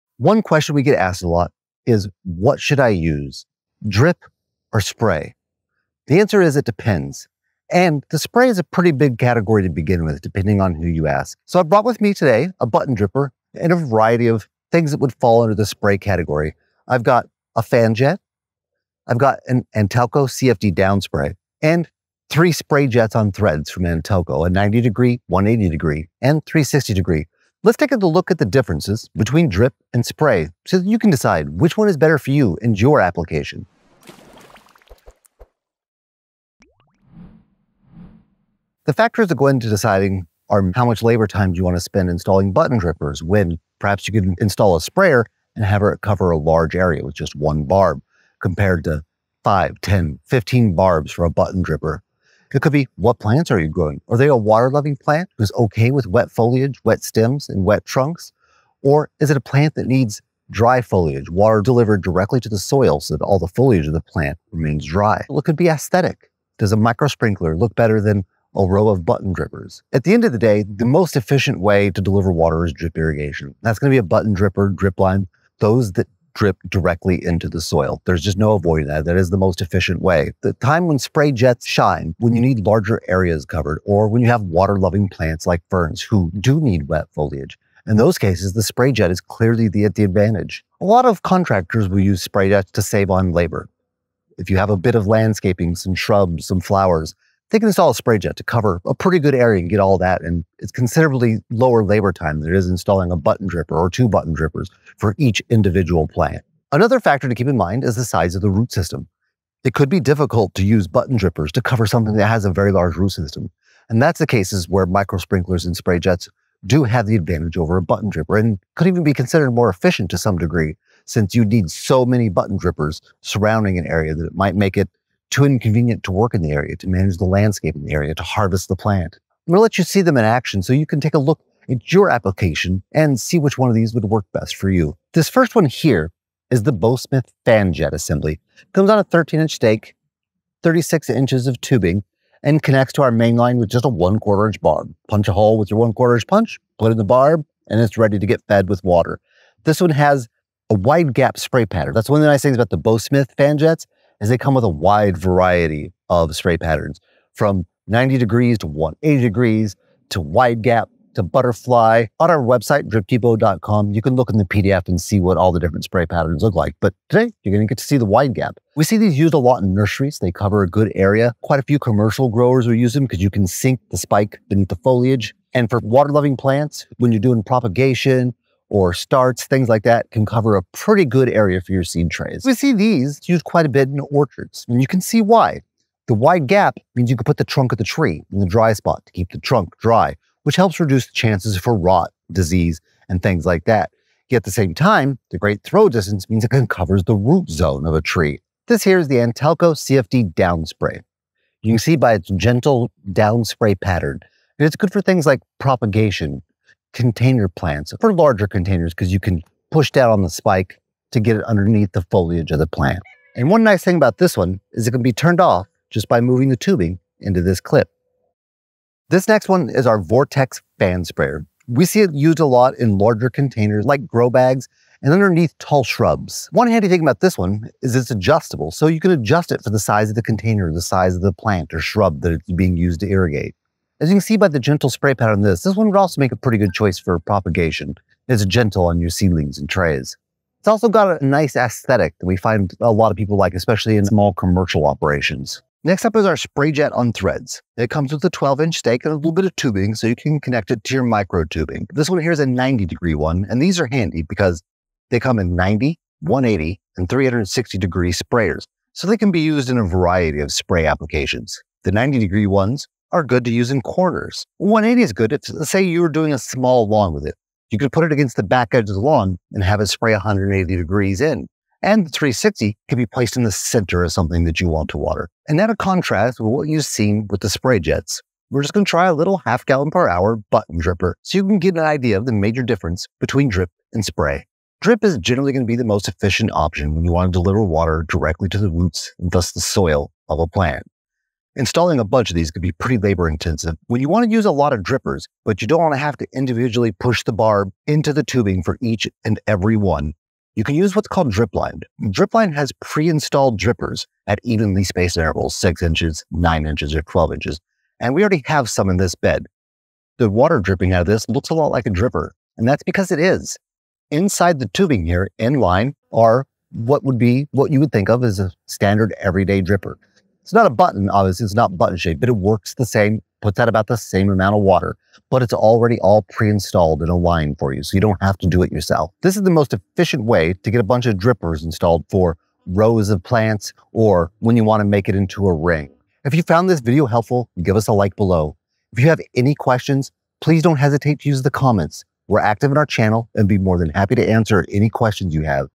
Spray vs. Drip Irrigation: What’s best for your Garden?
Drip or Spray emitters? Adam walks through the difference between Drip and Spray irrigation emitters and what to consider when determining the best choice for your garden or landscape.